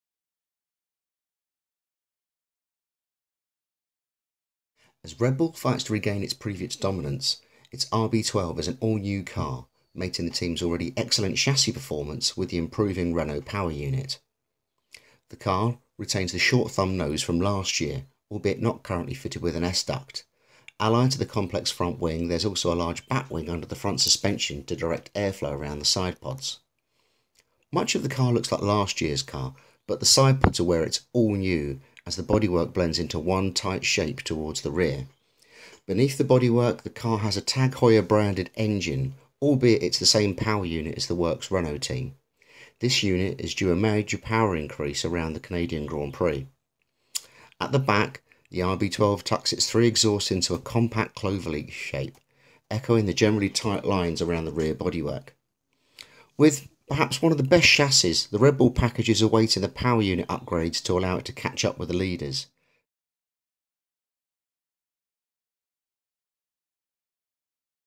RACER: F1 Red Bull 2016 Technical Insight
Take a technical tour the Red Bull RB12 chassis in RACER.com's new series of Formula 1 videos for the 2016 season.